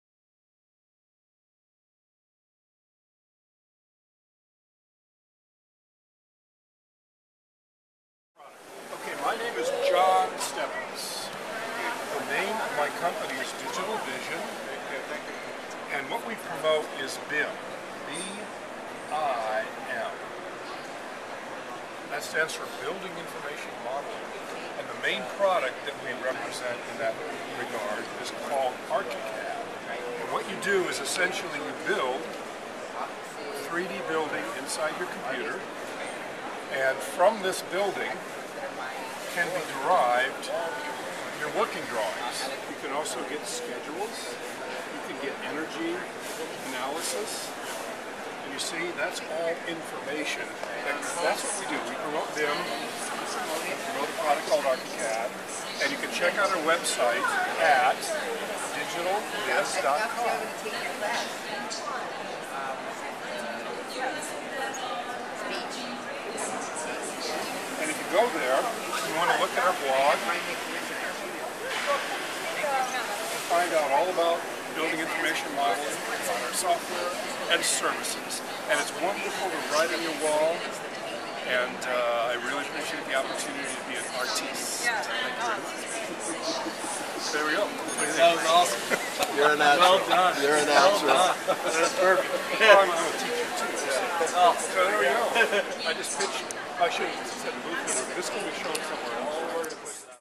IdeaPaint Pitch Your Product - John from Digital Vision Automation
John Stebbins from Digital Vision Automation pitches his product on IdeaPaint at Dwell  on Design '09 in LA.